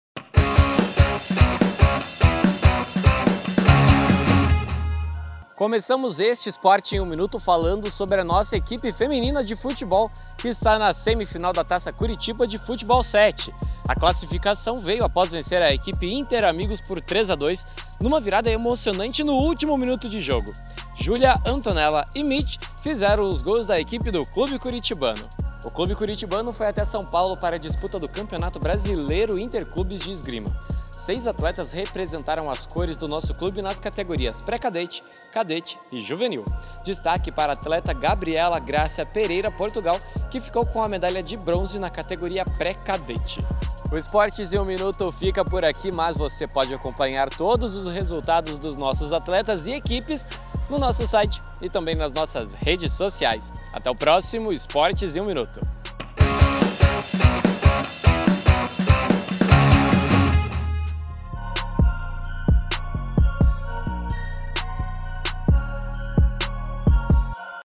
Futebol 7 e Interclubes de Esgrima - Esportes em 1 minuto | Clube Curitibano
Temas desta edição: Classificação da equipe feminina do Clube na Taça Curitiba Futebol 7 e pódio na Campeonato Brasileiro Interclubes de Esgrima.